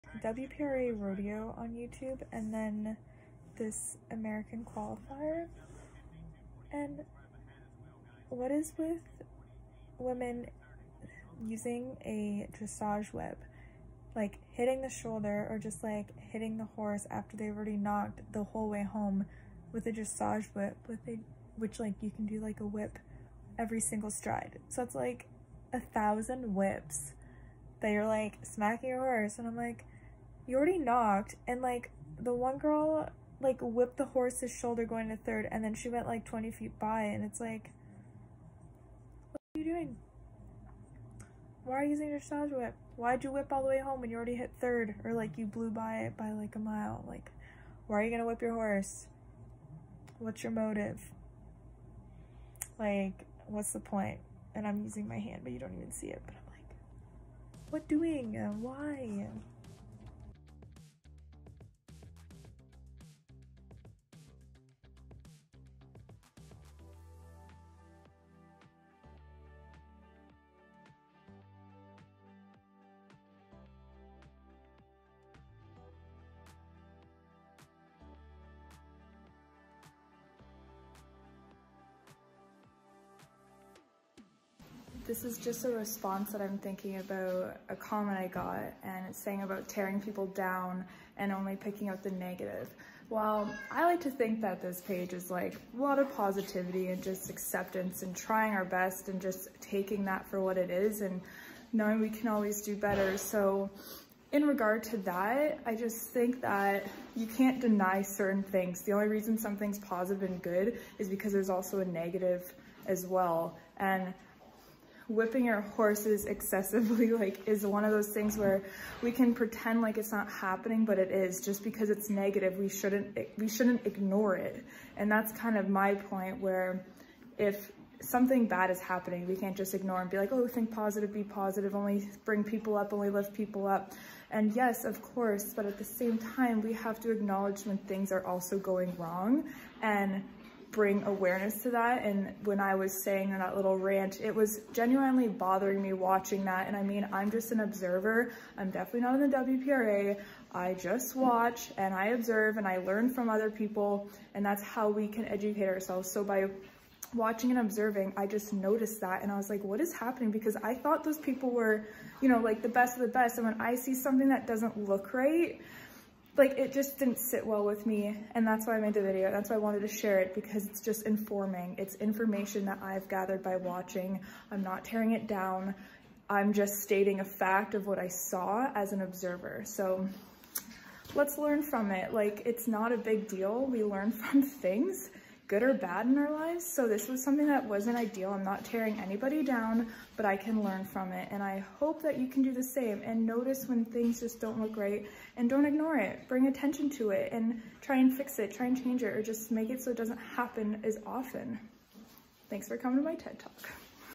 Reaction Video to my "Whipping your Horse 1000x Rant"
Response to why I decided to rant and share my observation from watching some big races and drayage whips in major action.

Let's do better, let's inform, bring awareness and learn as a collective!

Want support?